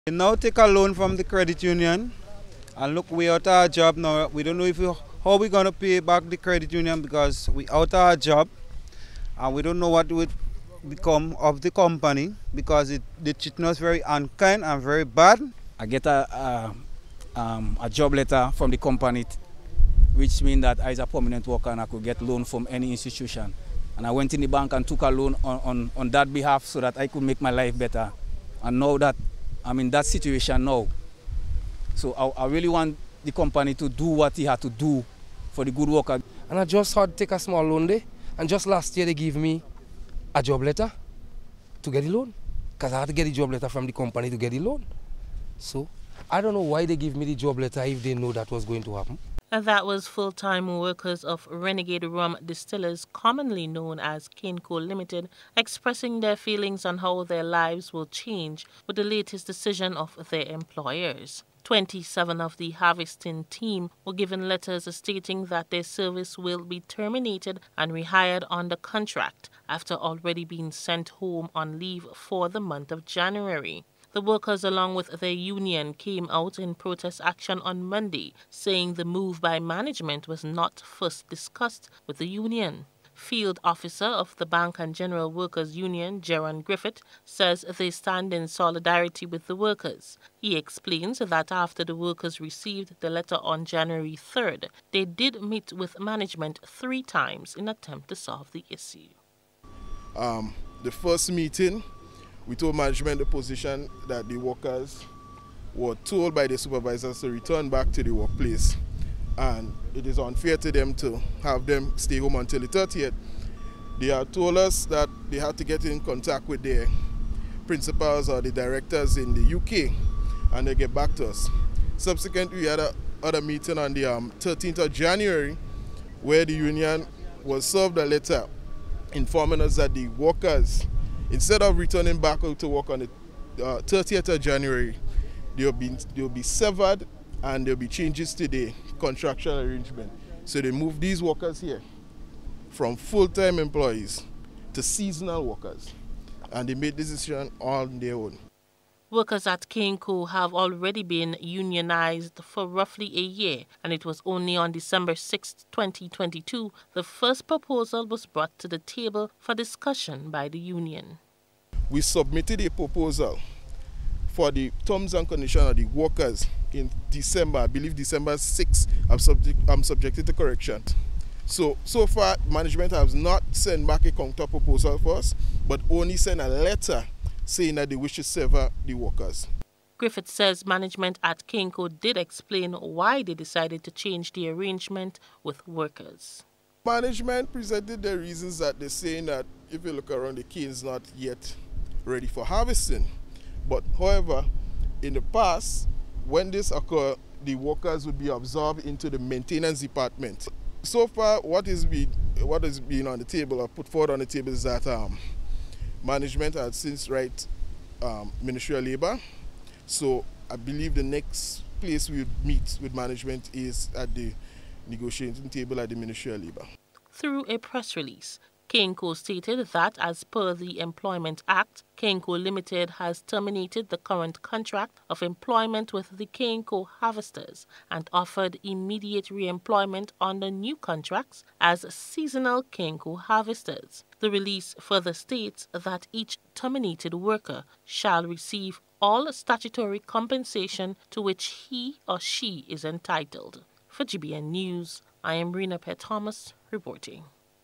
CANECO Workers Protest
Some workers of CANECO limited protested at the entrance of the business headquarters on Monday morning, opposing the termination of permanent cane harvesters.